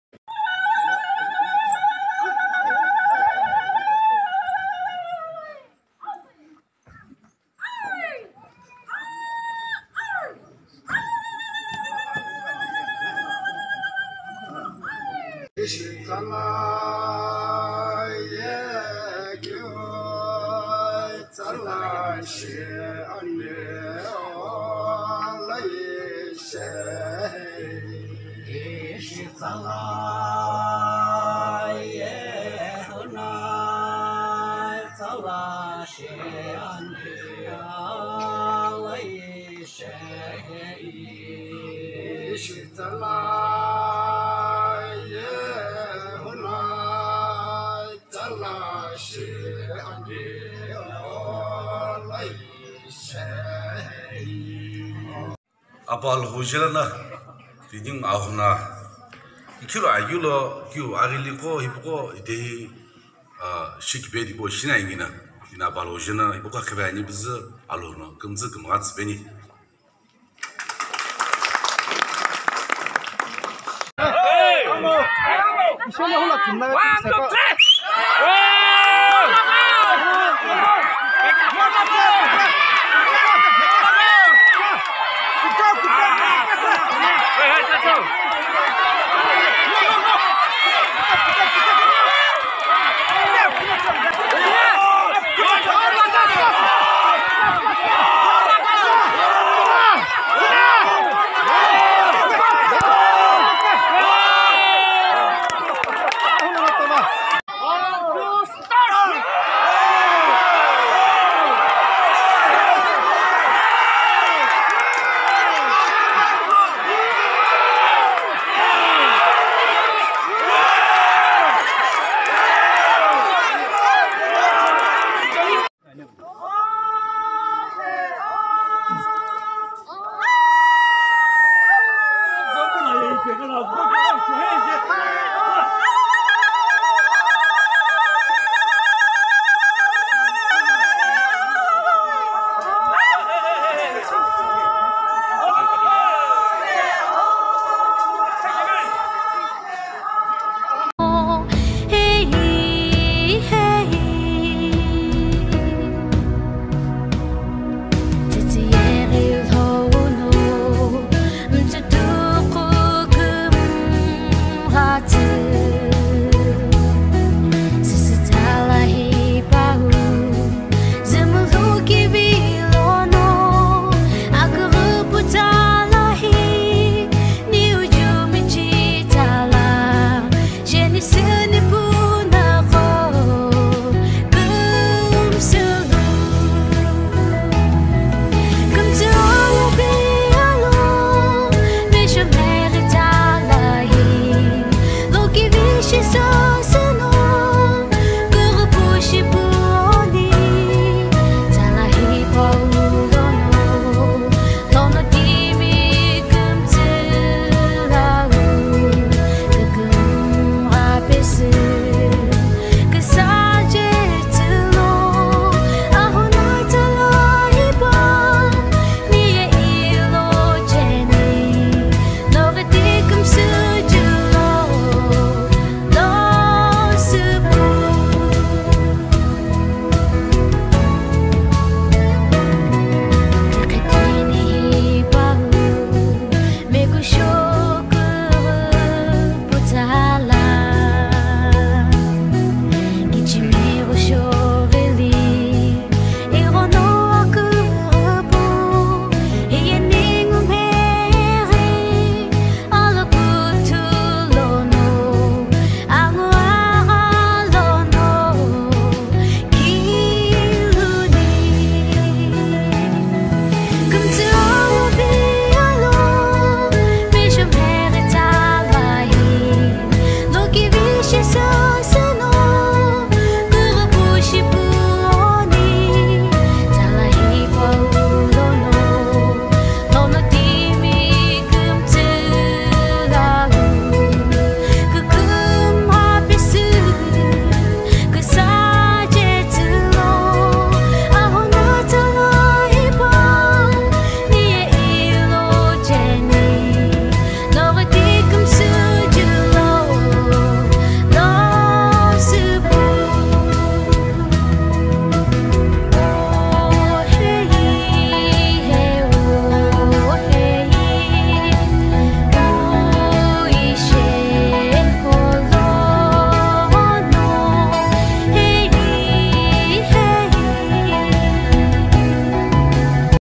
Celebrating Ahuna Festival... 🌾🌾at Shitoi Village.
Sumi people celebrating Ahuna Festival...🌾🌾 on 13 November 2021 at Shitoi Village.